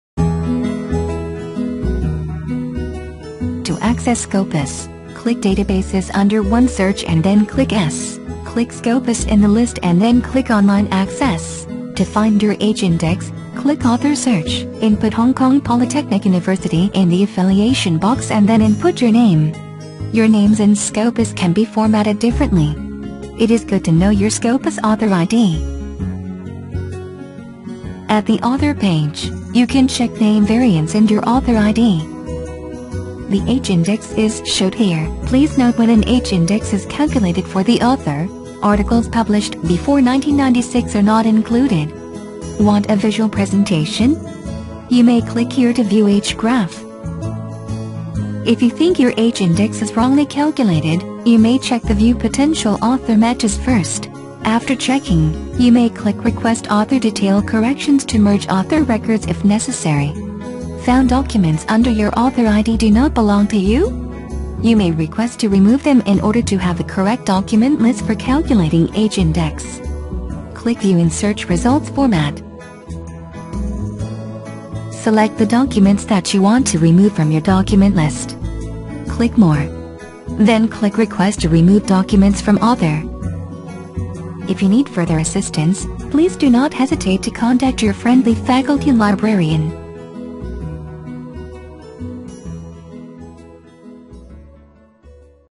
Finding h-index and Managing Author ID at Scopus
You will learn how to locate author's h-index at Scopus and manage author's publication list.